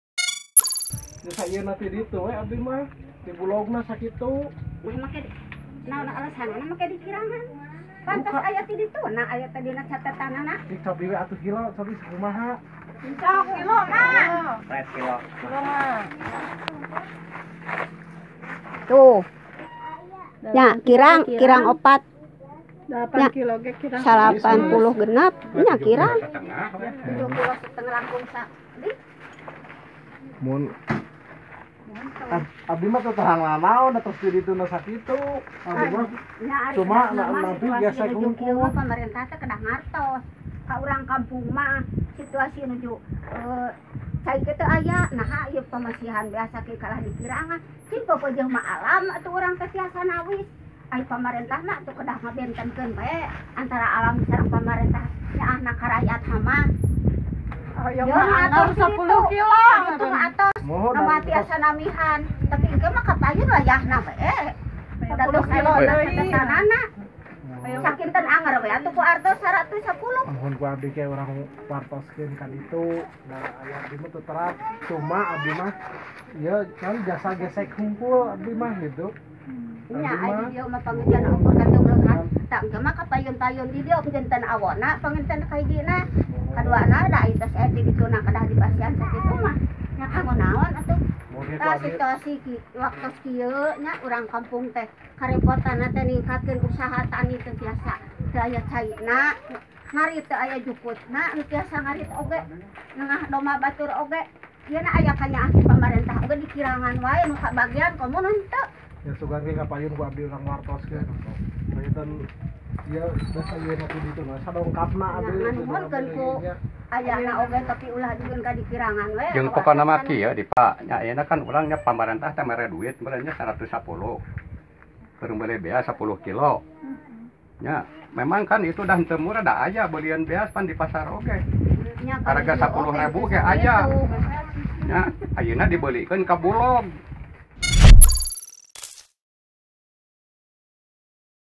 Heboh Nenek di Sumedang Kecewa Timbangan Beras BPNT Kurang
Heboh video warga penerima Bantuan Pangan Non Tunai (BPNT) Kabupaten Sumedang, Jawa Barat kecewa dengan jumlah beras yang diterimanya. nenek dalam video itu menyebut berat beras cuma kurang dari 10 kg.